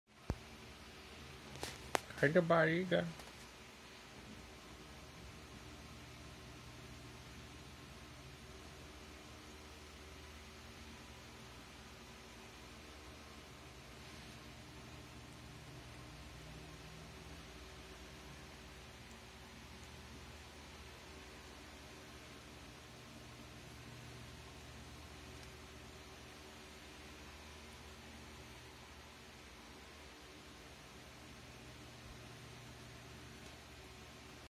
Gato Fredi brincando na hora de dormir
Aí galera assistam o vídeo do gato Fredi brincando na hora de dormir